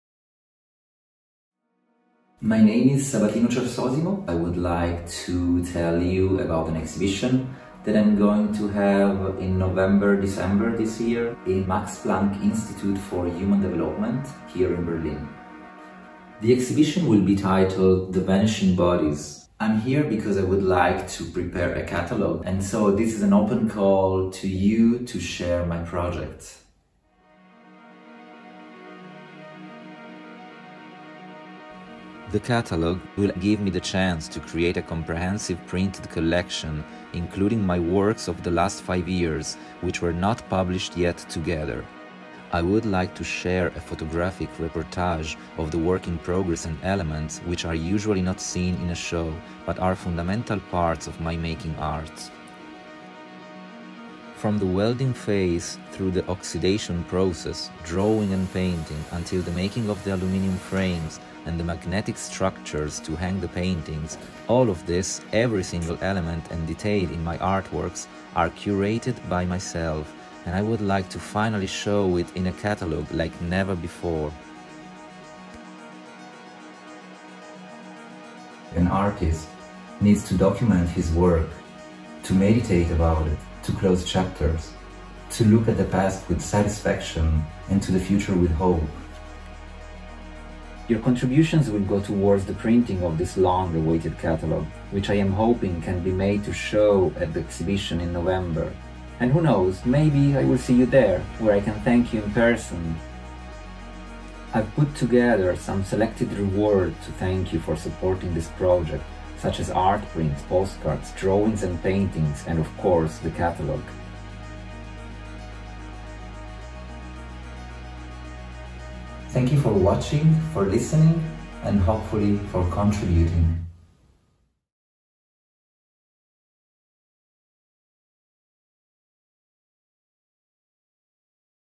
The Vanishing Bodies Crowdfunding Campaign
Support Contemporary Artist! :)

A little introduction to who I am and why I need your support to create a catalogue of my works for the exhibition "The Vanishing Bodies" at Max Planck Institute for Human Development in Berlin, November-December 2023.
You will see samples of how I create my paintings which are oil and oxidation on steel plates, and go through the welding phase, oxidation process, painting and framing.
The concepts of time, control and unpredictability  play the main roles in my paintings, which appear fragmented and, in fact, partly vanishing, as the title suggest.